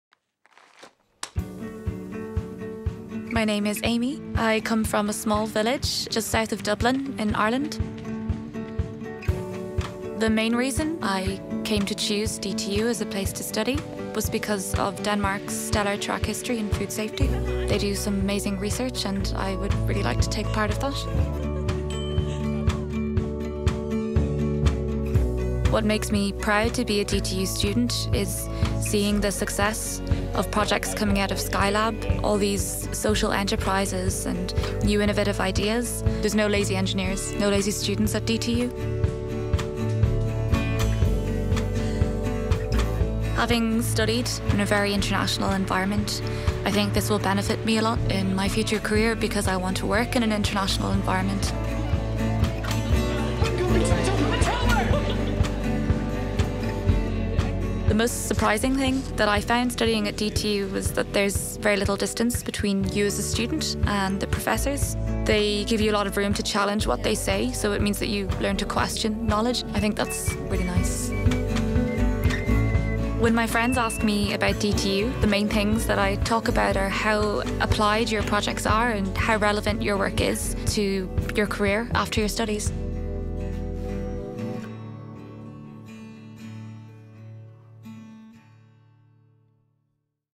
Study at DTU – Amy from Ireland
Amy Fitzpatrick is enrolled in the MSc in Food Technology at DTU.
Last semester, she was given a film camera and a mission: to tell prospective students what life is like at DTU.
Here is her film.